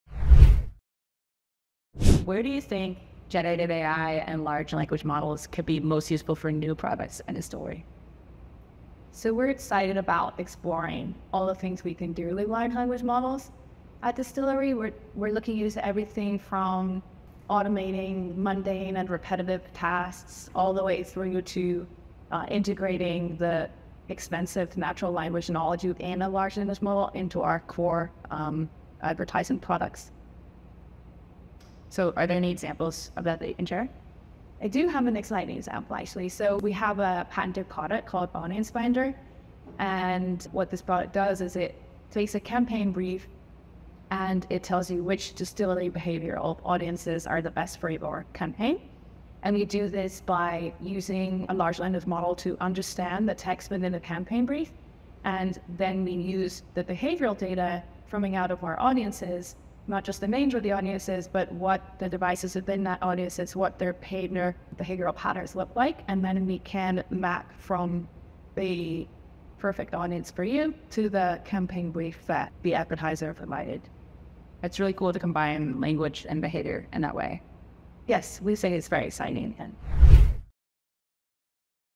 DST POV: How is Dstillery utilizing generative AI and large language models for innovation?
In this episode of DST POV, Dstillery's Chief Data Scientist, Melinda Han Williams, sits down with Dstillery’s VP of Data Science Research, Amelia White.

Amelia explains how Dstillery is utilizing generative AI and large language models to innovate in many different ways from automating tasks to developing groundbreaking targeting technologies.